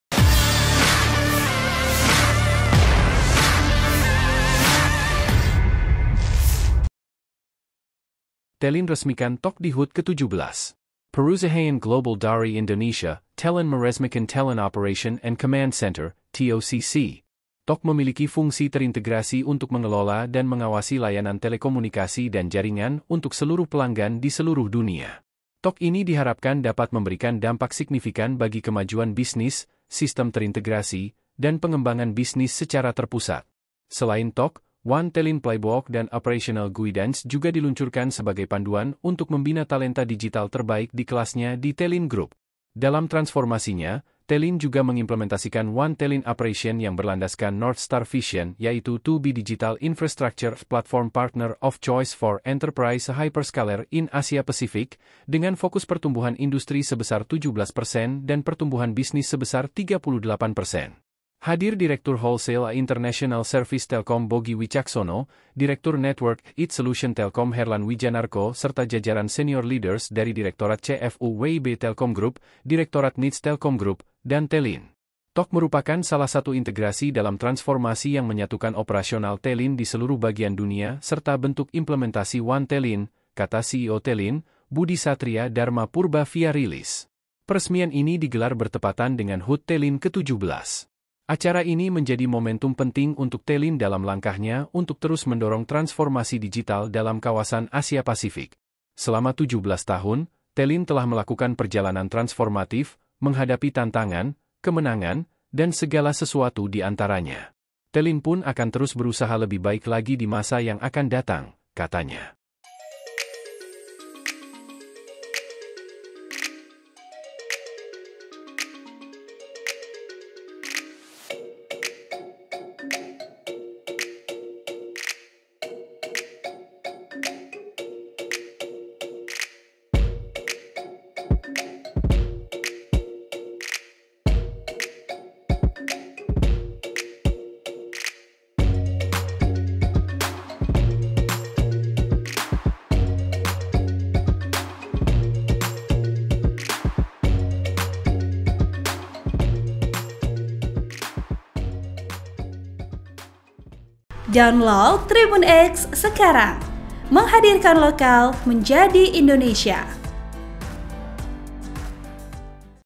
ONE Telin Playbook Membina Talenta Digital
TRIBUN-MAKASSAR - Perusahaan global dari Indonesia, Telin meresmikan Telin Operation and Command Center (TOCC).

TOCC memiliki fungsi terintegrasi untuk mengelola dan mengawasi layanan telekomunikasi dan jaringan untuk seluruh pelanggan di seluruh dunia.

TOCC ini diharapkan dapat memberikan dampak signifikan bagi kemajuan bisnis, sistem terintegrasi, dan pengembangan bisnis secara terpusat.

Selain TOCC, ONE Telin Playbook dan Operational Guidance juga diluncurkan sebagai panduan untuk membina talenta digital terbaik di kelasnya di Telin Group.

Dalam transformasinya, Telin juga mengimplementasikan ONE Telin Operation yang berlandaskan North Star Vision yaitu To Be Digital Infrastructure & Platform Partner of Choice for Enterprise & Hyperscaler in Asia Pacific, dengan fokus pertumbuhan industri sebesar 17 persen dan pertumbuhan bisnis sebesar 38 persen.

Hadir Direktur Wholesale & International Service Telkom Bogi Witjaksono, Direktur Network & IT Solution Telkom Herlan Wijanarko serta jajaran Senior Leaders dari Direktorat CFU WIB TelkomGroup, Direktorat NITS TelkomGroup, dan Telin.

"TOCC merupakan salah satu integrasi dalam transformasi yang menyatukan operasional Telin di seluruh bagian dunia serta bentuk implementasi ONE Telin,"  kata CEO Telin, Budi Satria Dharma Purba via rilis.

Peresmian ini digelar bertepatan dengan HUT Telin ke-17.

Acara ini menjadi momentum penting untuk Telin dalam langkahnya untuk terus mendorong transformasi digital dalam kawasan Asia-Pasifik.

Selama 17 tahun, Telin telah melakukan perjalanan transformatif, menghadapi tantangan, kemenangan, dan segala sesuatu di antaranya.

"Telin pun akan terus berusaha lebih baik lagi di masa yang akan datang," katanya.(*)

Video: Muhammad Abdiwan